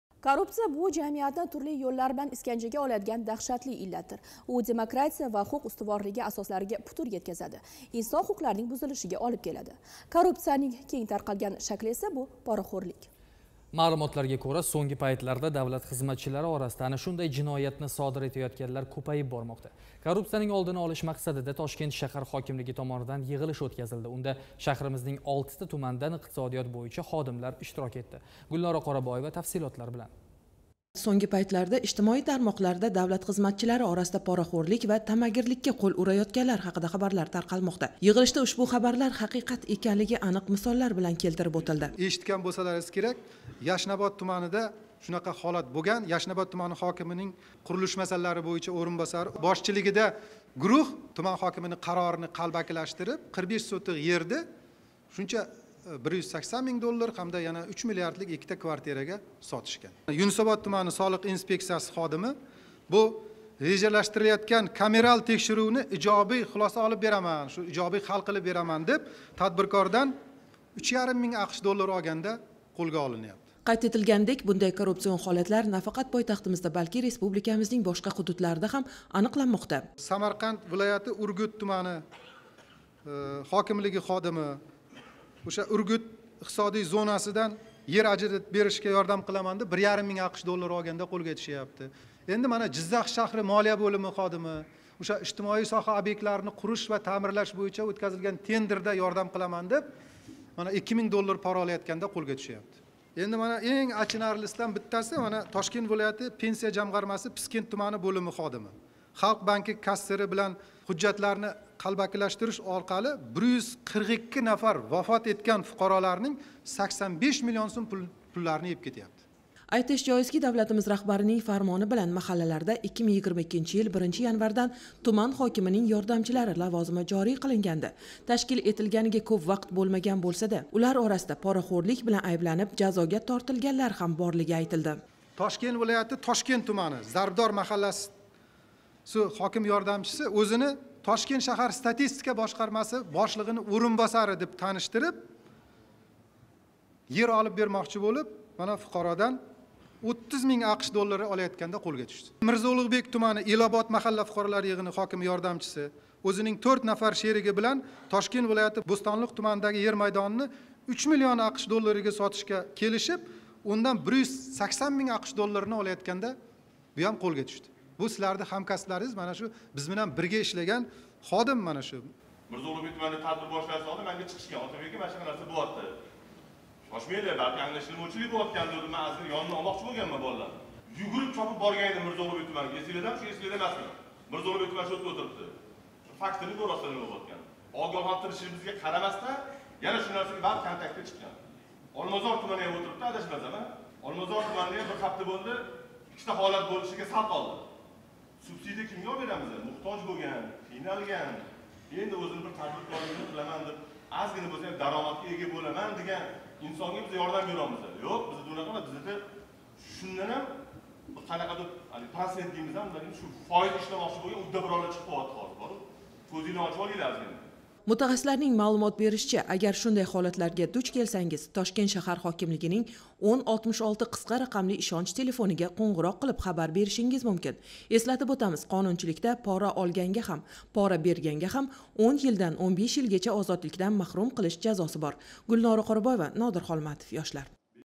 Пора билан ушланган давлат хизматчилари
Пенсия жамғармаси ва Халқ банки ходимлари вафот этган 142 нафар шахсларнинг 85 миллион сўмдан ортиқ пулини ўзлаштирган.